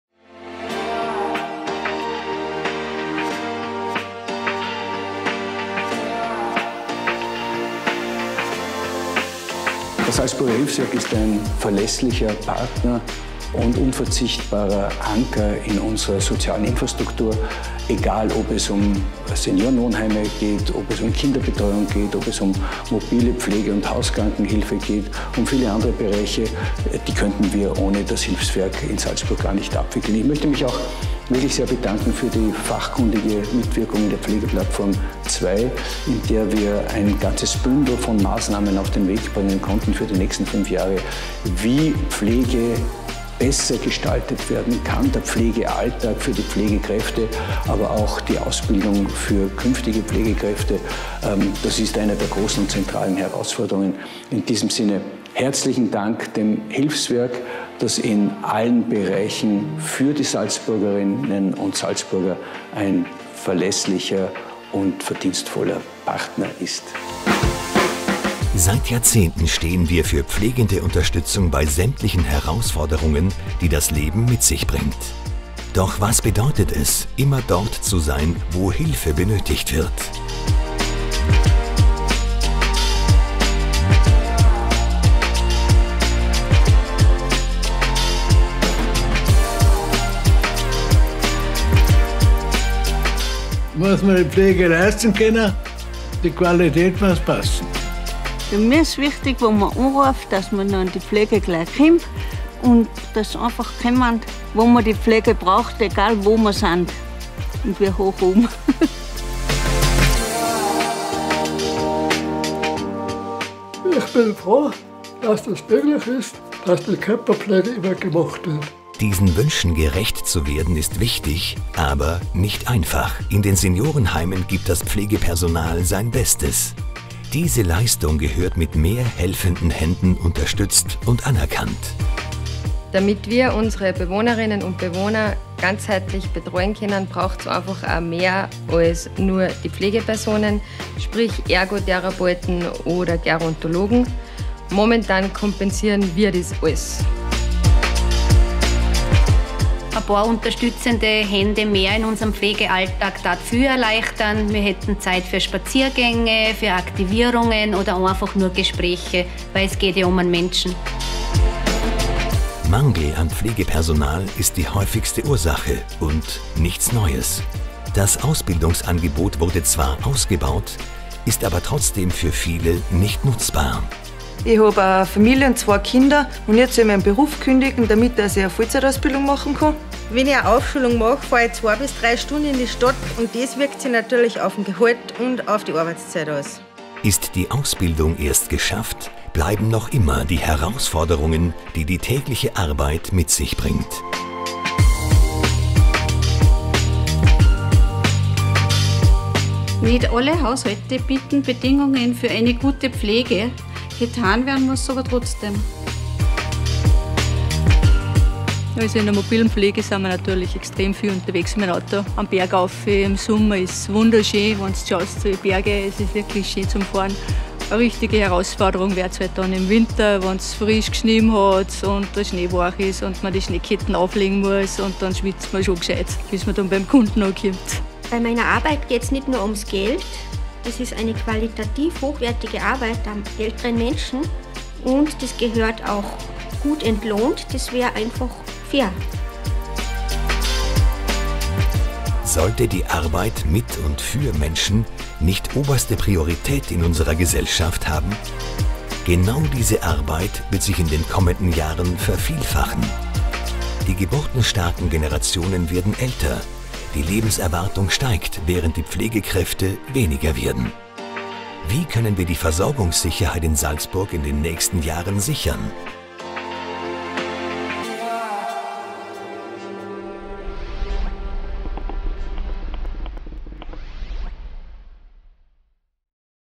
Hilfswerk Salzburg - Anforderungen Pflege mit Statement Landeshauptmann Wilfried Haslauer
Im Rahmen der Generalversammlung des Hilfswerks Salzburg wurde ein Video zu den Anforderungen, welche in der Pflege im Bundesland notwendig sind. Landeshauptmann Wilfried Haslauer nahm dazu Stellung.